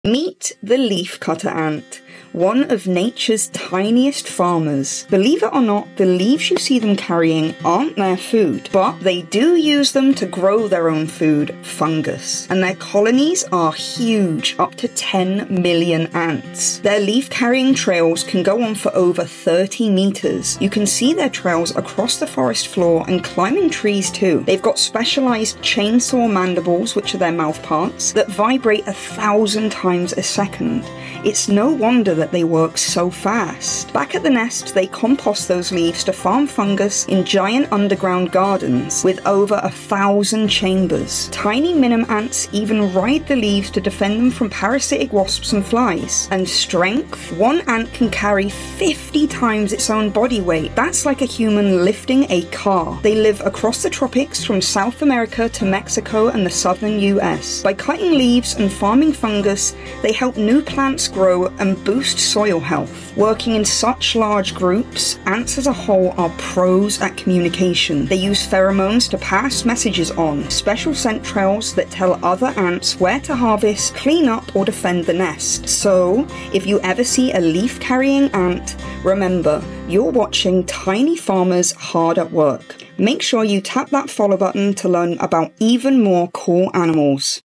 The Ants That Grow Their Own Food
Leafcutter ants don't just cut leaves - they farm fungus, build massive underground colonies, and communicate using scent trails. With jaws that vibrate like chainsaws and strength that rivals a human lifting a car, these ants are some of the most fascinating insects on Earth!